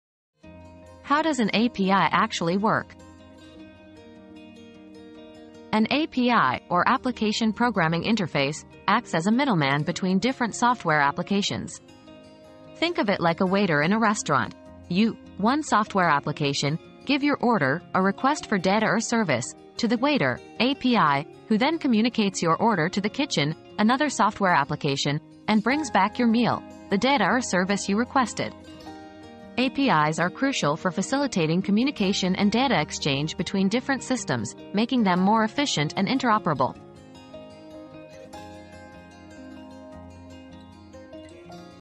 How does an API actually work?
Unveiling the Magic of APIs: How They Actually Work • Decoding APIs: How They Work • Discover the inner workings of APIs and their role as digital middlemen, enabling seamless communication and data exchange between software applications. Join us as we demystify APIs by comparing them to a restaurant experience, where they act as efficient waiters connecting your requests to the desired software applications and delivering the data or service you need. Learn how APIs make systems more interoperable and enhance overall efficiency.